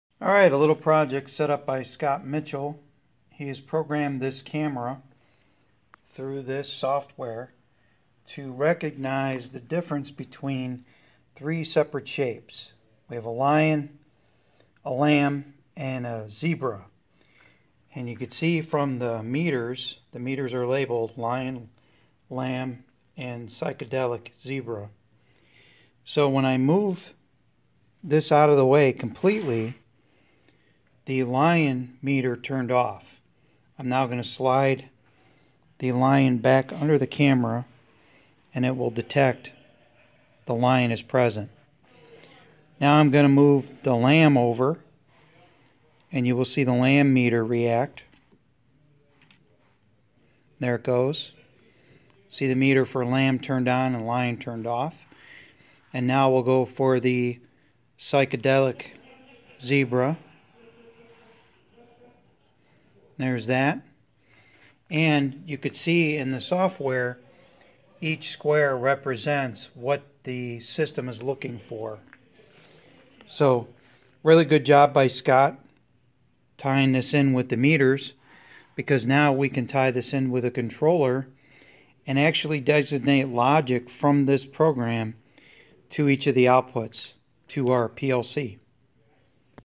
Robotic Vision System
Second-year student Scott has learned how to program this Vision system to recognize different shapes and characters. The Next Step will be to give the system some decision making capabilities to outside machines so the physical world can adapt to different parts.
Sensopart V10 Camera